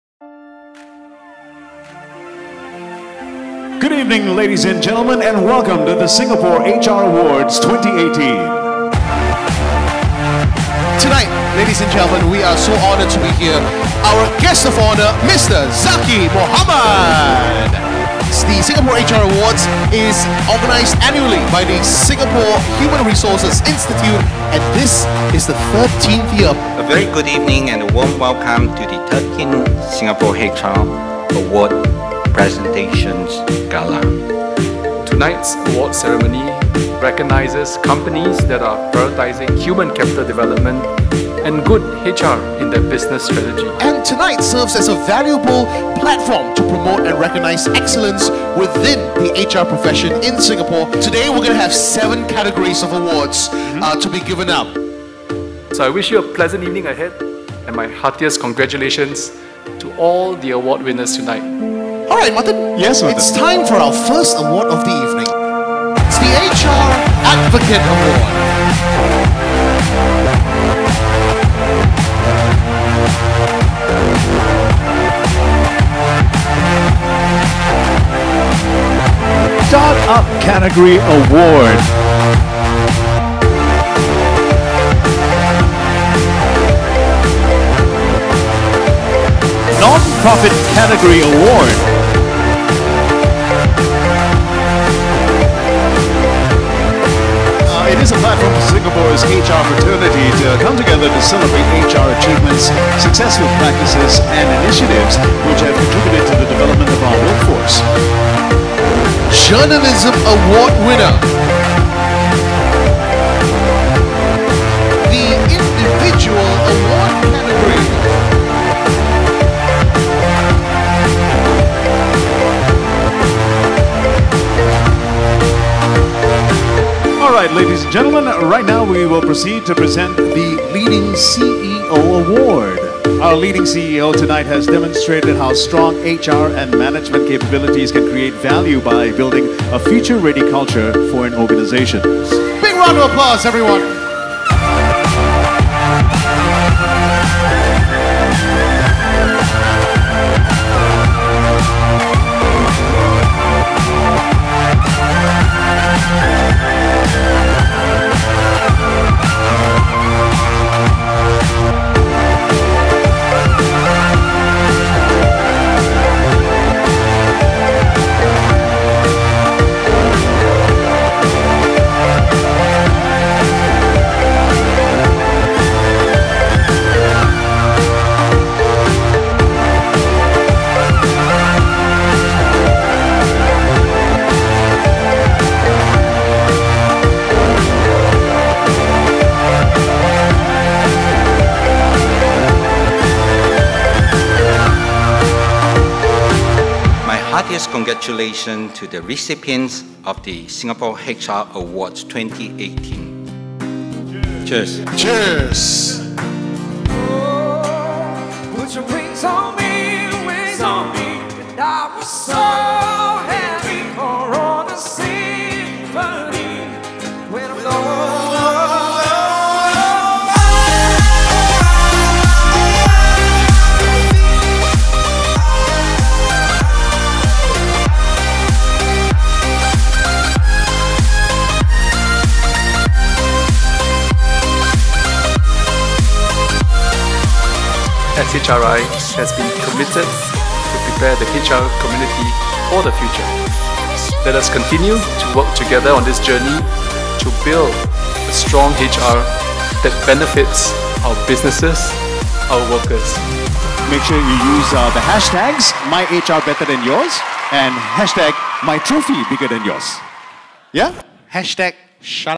Singapore HR Awards 2018 Highlight Reel
Organised annually by Singapore Human Resources Institute (SHRI) and in its 13th year, The Singapore HR Awards celebrate leading organisations and HR professionals in their drive for impactful human capital strategies. It serves as a valuable platform to promote and recognise excellence within the HR profession in Singapore by highlighting leading, innovative organisational HR practices and individuals that have greatly impacted the quality of organisations and the workforce.

The event was graced by Mr Zaqy Mohamad, Minister of State, Ministry of National Development & Ministry of Manpower, as Guest-of-Honour, and attended by 500 guests comprising HR professionals, employers, business leaders, media and overseas partners.

This year, 60 awards were presented to journalists, start-ups, not-for-profits, public and private sector organisations with exemplary people management practices, across various sectors such as healthcare, education, construction, transport, telecommunications, IT, hospitality and many more.